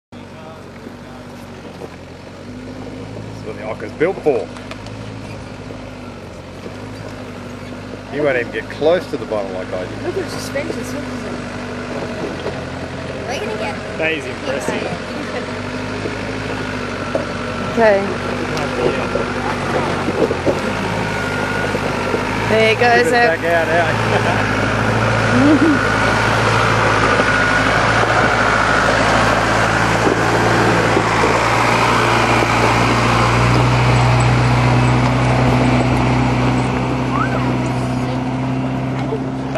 Gunbarrel Highway, climbing Mt Nossiter in OKA 260
Mount Nossiter is a 519m-high peak just off the Gunbarrel Highway, 104km east of Carnegie Station in the Little Sandy Desert, central-eastern Western Australia. This little bit is steeper than it looks in the video!